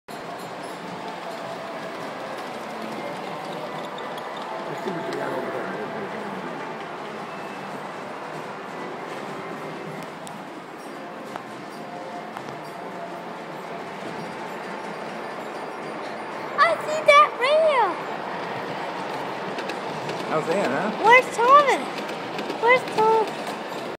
Festival of the Trains #4
Traverse City train show at the History Center